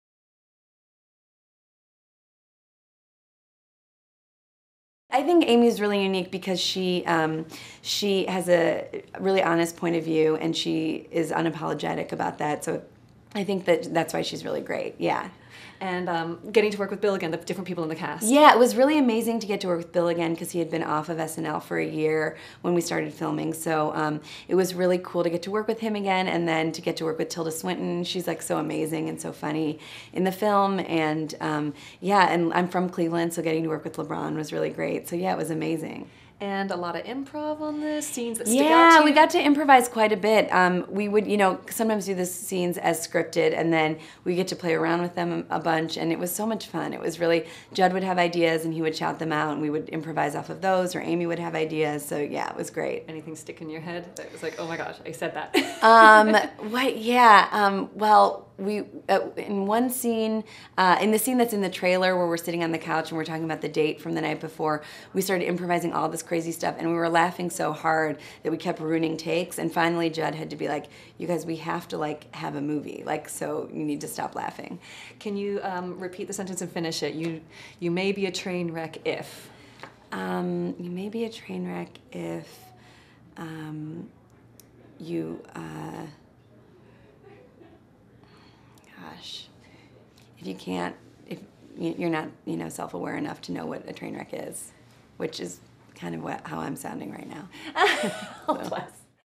Vanessa Bayer - Trainwreck Interview HD
Vanessa Bayer talks about working with Amy Schumer, Bill Hader and Tilda Swinton in 'Trainwreck' and admits that she enjoyed when the cast was allowed to improvise.

Trainwreck movie synopsis:

A magazine writer (Amy Schumer) who was raised to believe that monogamy isn't realistic finds herself falling for the subject of the article she's writing, a successful sports doctor (Bill Hader).

Starring: Amy Schumer, Bill Hader, Brie Larson, Colin Quinn, John Cena, Vanessa Bayer, Mike Birbiglia, Ezra Miller, Dave Attell, Tilda Swinton, LeBron James

In theatres: July 17, 2015